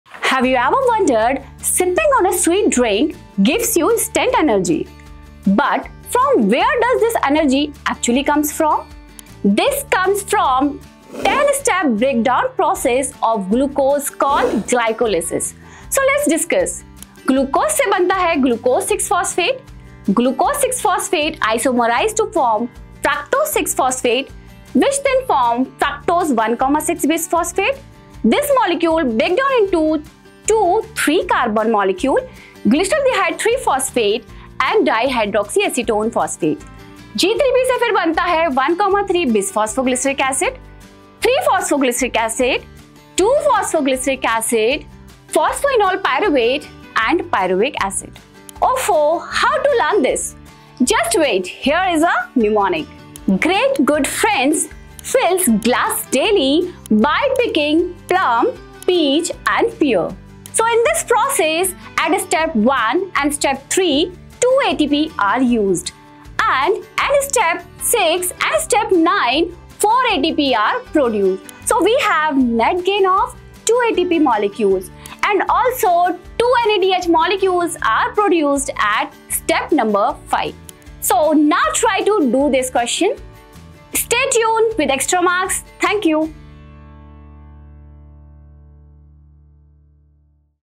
Glycolysis in 10 Easy Steps | NEET Biology | NEET 2026
Unlock the secrets of Glycolysis with Extramarks’ engaging mnemonic! Perfect for NEET aspirants, this video breaks down the 10-step glucose breakdown process into simple, memorable steps. Learn how glucose transforms into pyruvate, the preparatory and payoff phases, ATP production, and NADH formation. 📚 Boost your NEET Biology prep with our fun "friends making juice" mnemonic!

🔥 Key Highlights:
10 steps of Glycolysis explained
Preparatory phase (2 ATP consumed)
Payoff phase (4 ATP produced, net 2 ATP)
2 NADH molecules at step 5
What happens to pyruvate after glycolysis?